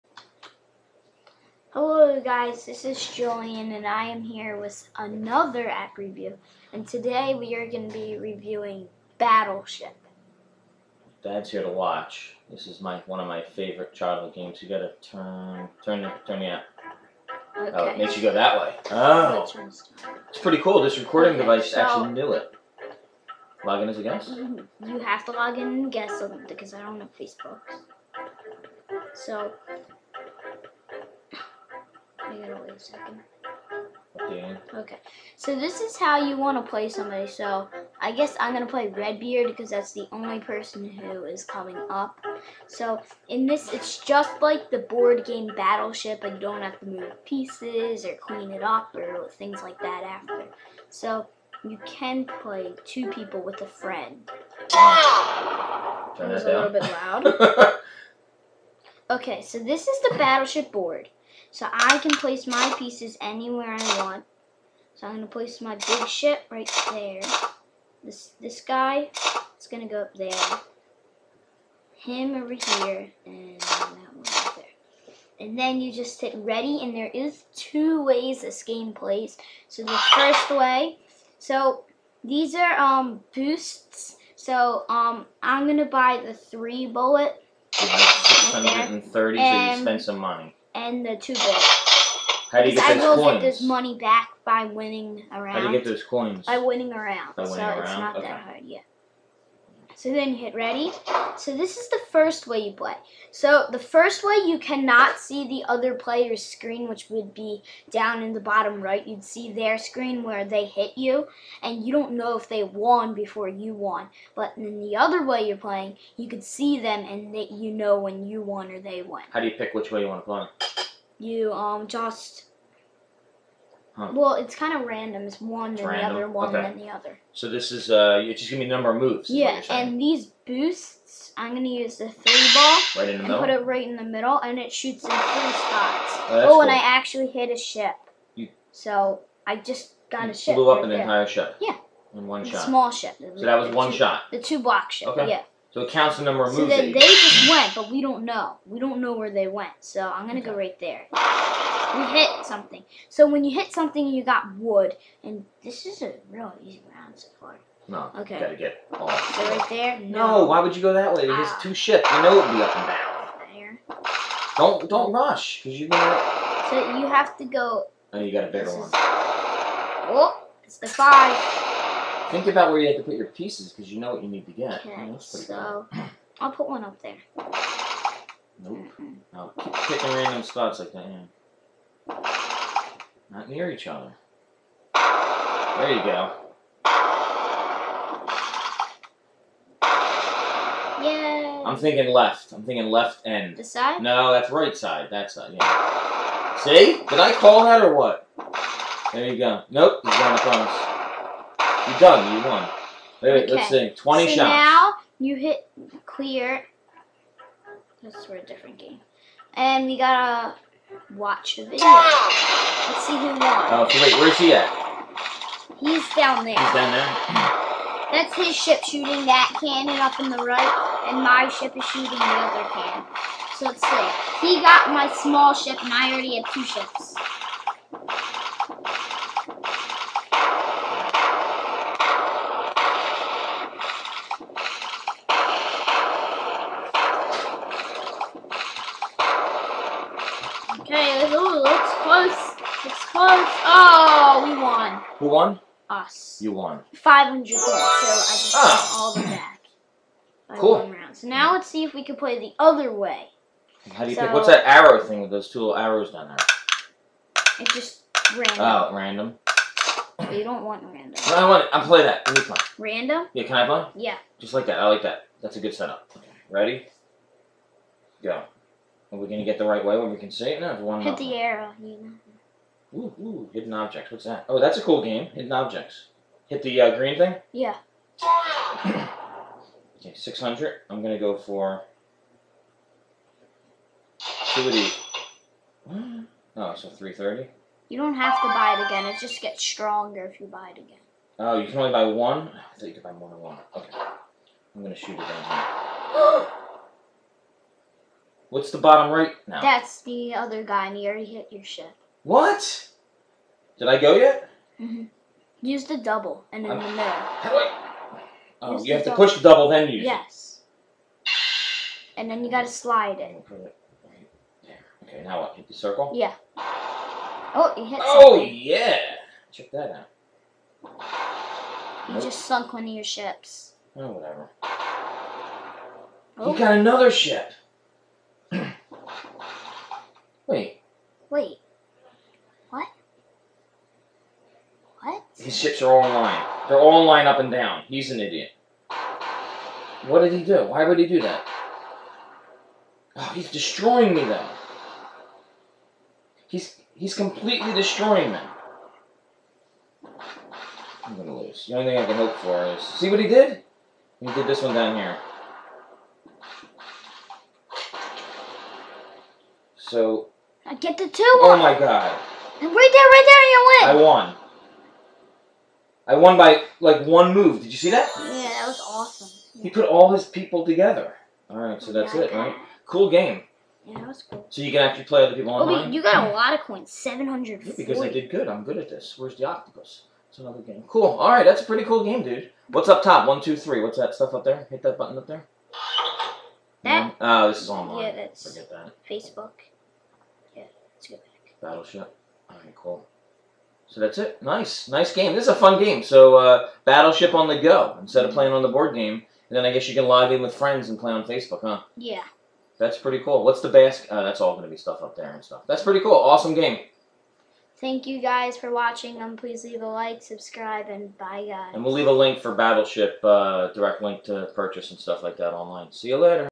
Battleship Shooter Game App Review - iPhone/iPod Touch/iPad
Julian, age 10 review the Battleship Shooter app game by Top Free Games.